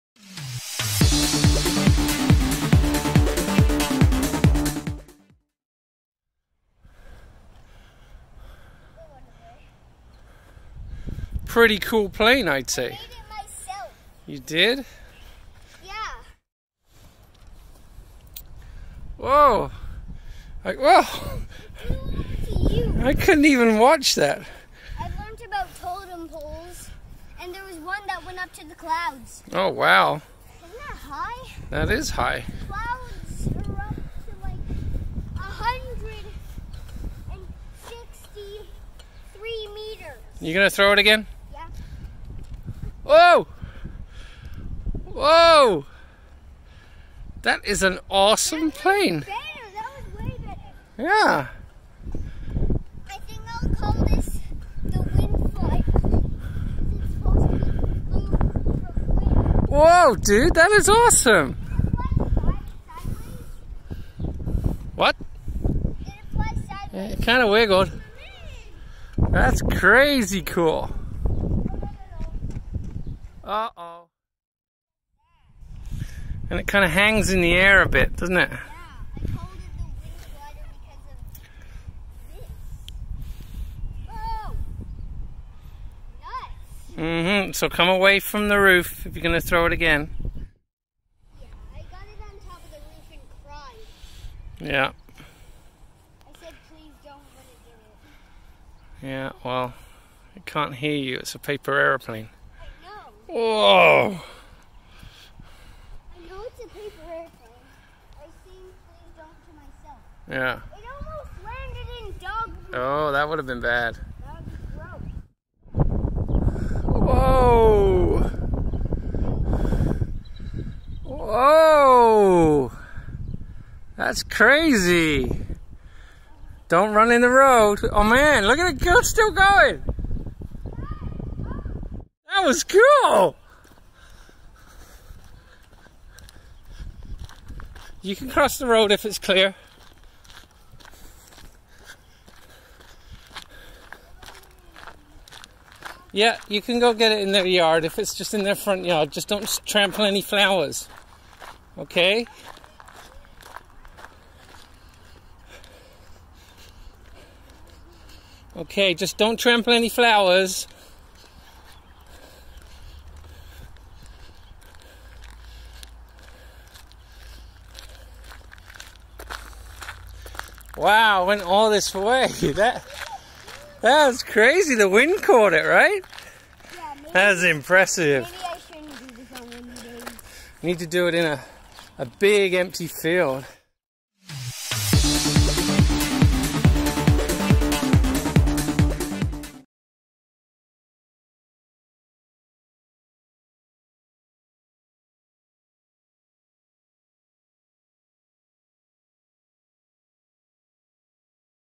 Paper Airplane - The Wind Flier - Test Flights and it really goes!!
Ethan loved folding and we buy origami paper from the dollar store to encourage his creativity,  paper planes are a favorite,  he bought him a pack of plain paper for Christmas and the majority of it became Plane Paper.  We ended up finding planes all around the house and still do sometimes.

He found this one he made at school in his nook so he brought it home and on our walk through the Luxton grounds he was throwing it around,  I was impressed so much I pulled out my phone and began filming and I was glad I did as it went an amazing distance!!

His folding has included origami boxes,  corner page bookmarks, envelopes and more,  he likes making it for others too and his school has encouraged his folding passion,  at valentines day he made paper boxes and we put cookies in them for all his class friends.

In a lot of our videos we will add comments relating to parenting challenges with our High Functioning ASD Kid.  Ethan!